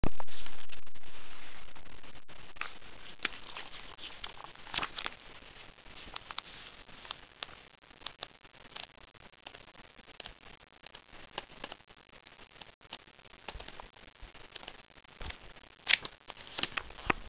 Book Review 3 ' Tales from Old Tonbridge' by Frank Chapman
This is a 'Bible' for local history fanatics. It also has a pic of the Russian Prince Belosselsky  (see his grave on one of my vid). It was good to get more info on him.There are pictures of the POW camp at Somerhill, the workhouse at Bank Street, and of the 'Venetion fetes' held along the river. Pity we don't have these now. The sooner we have a museum the better. Tonbridge was exciting in the old days!